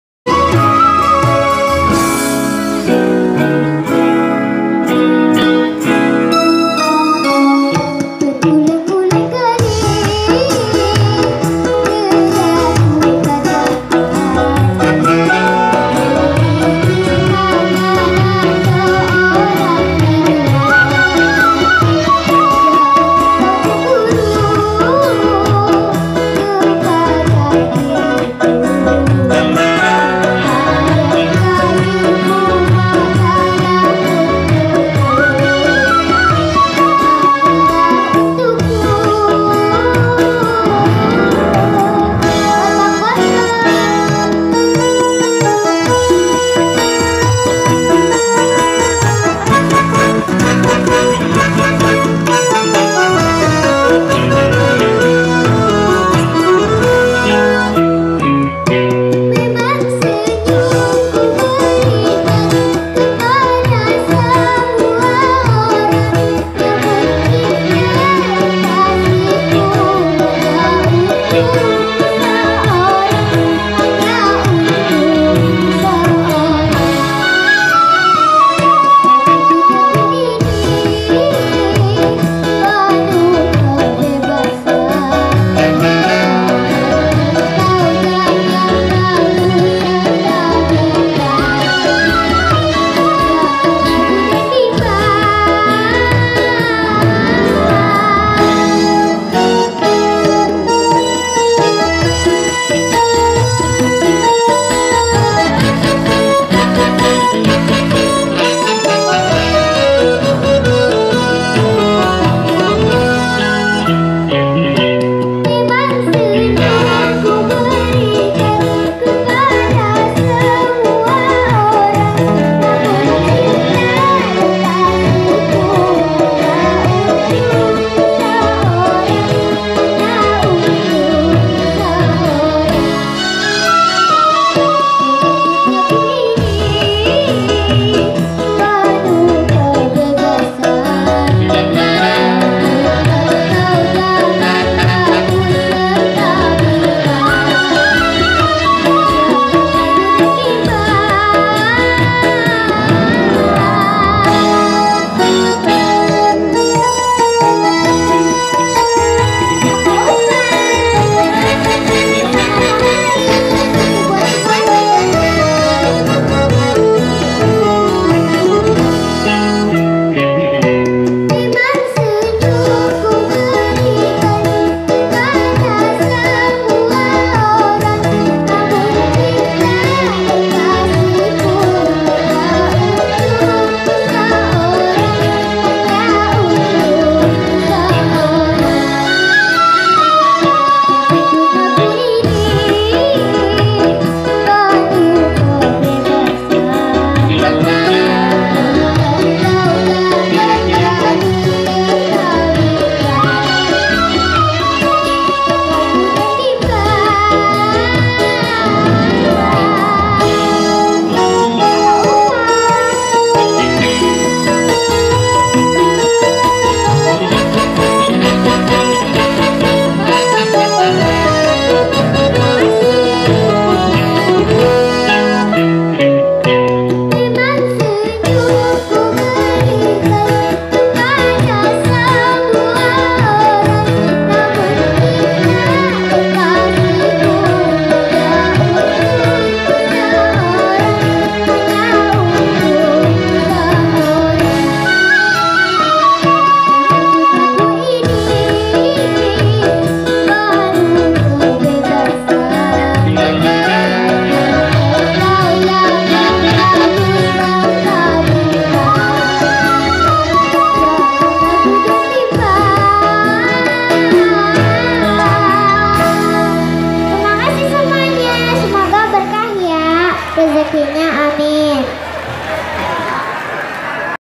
Si kecil cabe rawit yang viral
Denniz Sansabilla gadis kecil yg viral karena iseng nyanyi pake baju tidur dengan suara bagus